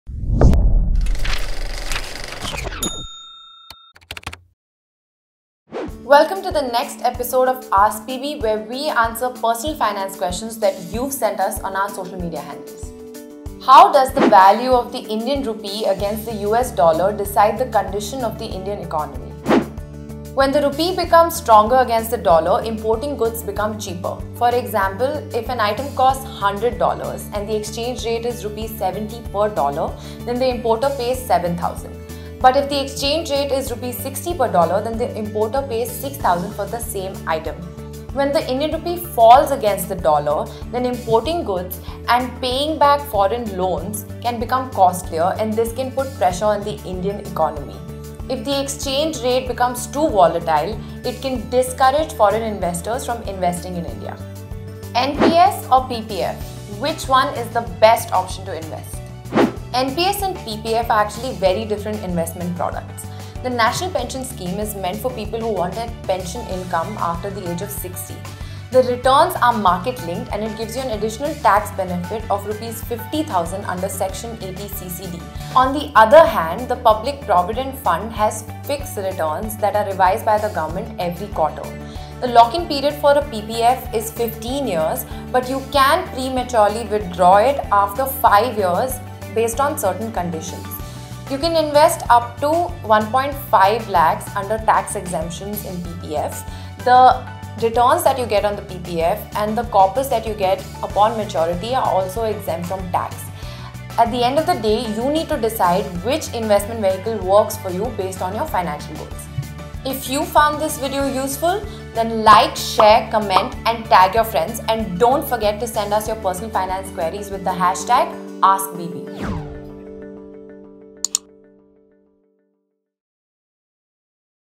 AskBB - NPS or PPF And Rupee Vs Dollar
In this episode, we explain the difference between the National Pension Scheme and the Public Provident Fund, and how the Indian Rupee's performance against the US Dollar affects our economy.